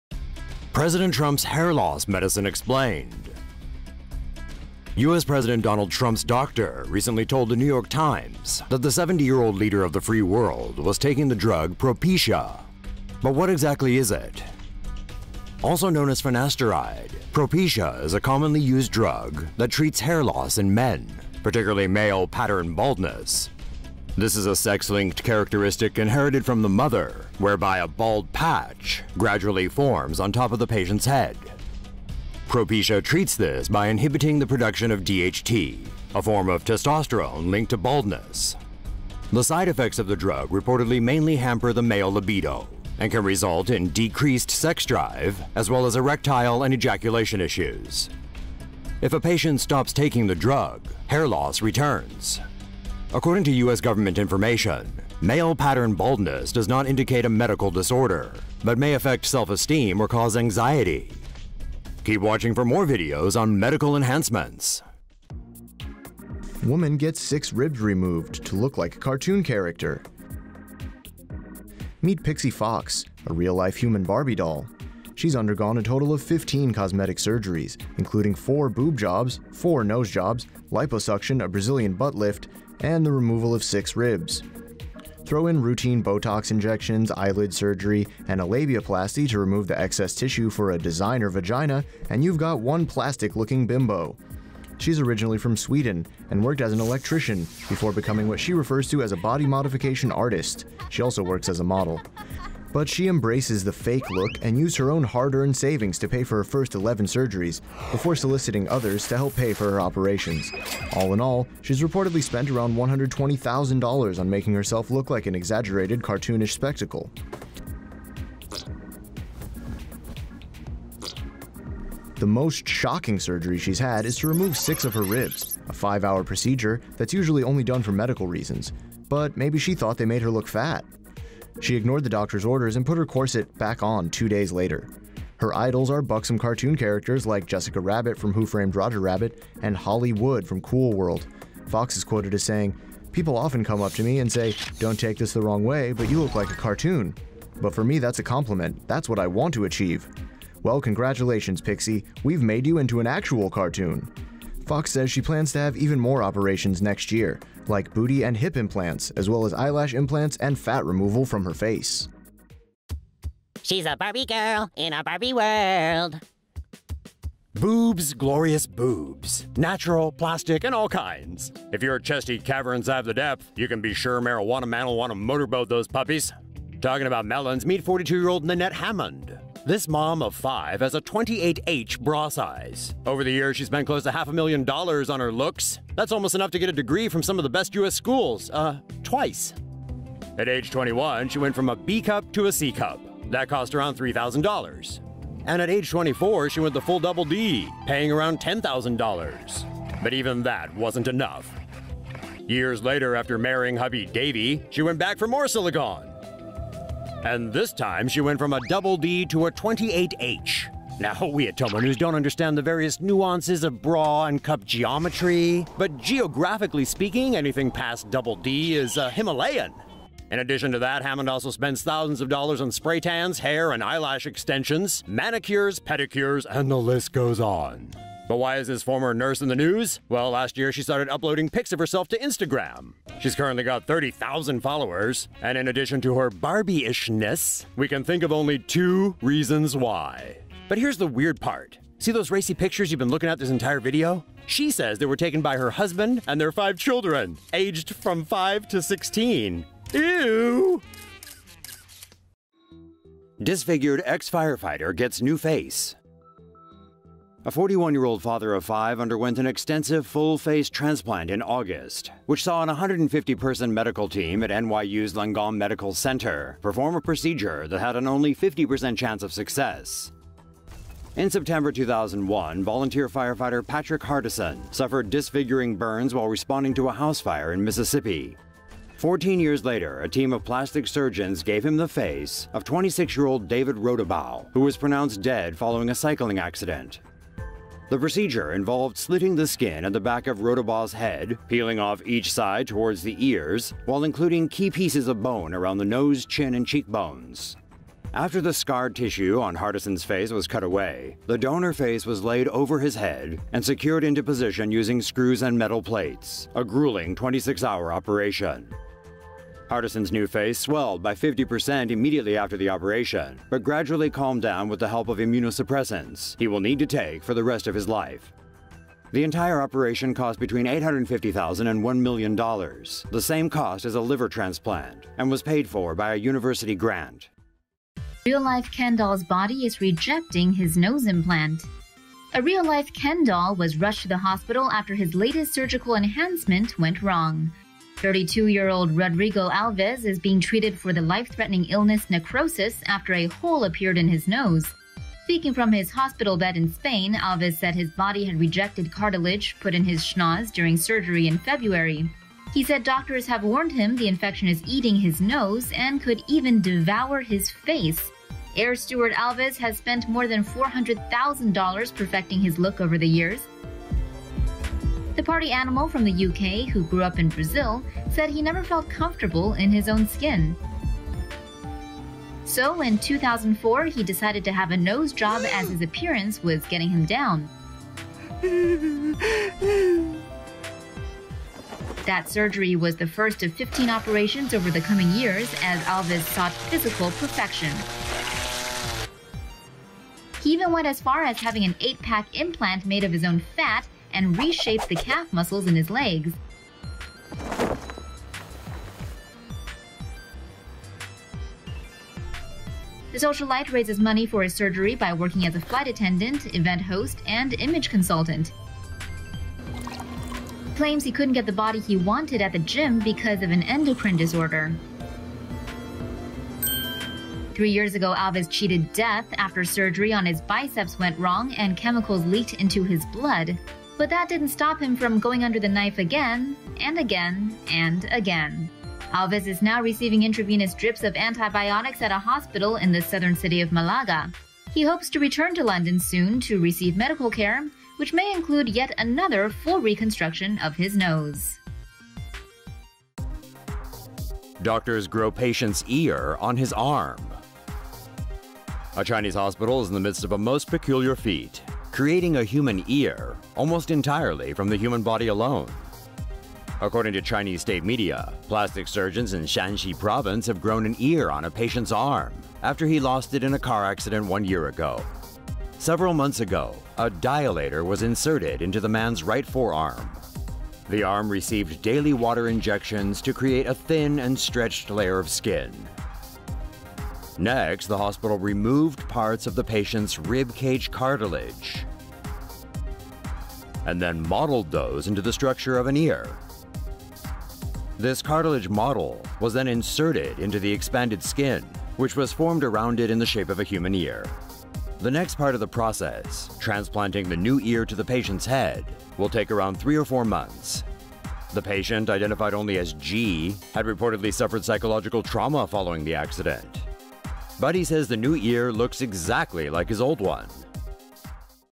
What is Propecia? President Donald Trump’s hair loss drug and its side effects explained - TomoNews
NEW YORK — U.S. President Donald Trump’s doctor recently told the New York Times that the 70-year-old was taking the drug Propecia.

Also known as finasteride, propecia is a commonly used drug that treats hair loss in men, particularly male-pattern baldness.

According to U.S. government information obtained from Mediline.gov, male-pattern baldness is a sex-linked characteristic inherited from the mother, whereby a bald patch gradually forms on top of a man’s head. Propecia treats this by inhibiting the production of DHT, a form of testosterone linked to baldness.

According to medicine information website Drugs.com, the side effects of Propecia mainly hamper the male libido and can result in decreased sex drive, as well as erectile and ejaculation issues. If a patient stops taking the drug, hair loss resumes.

Male pattern baldness does not indicate a medical disorder, but may affect self-esteem or cause anxiety.

New York physician Harold Bornstein told the New York Times that President Trump takes medicine to control his cholesterol and antibiotics to treat rosacea, a common skin condition.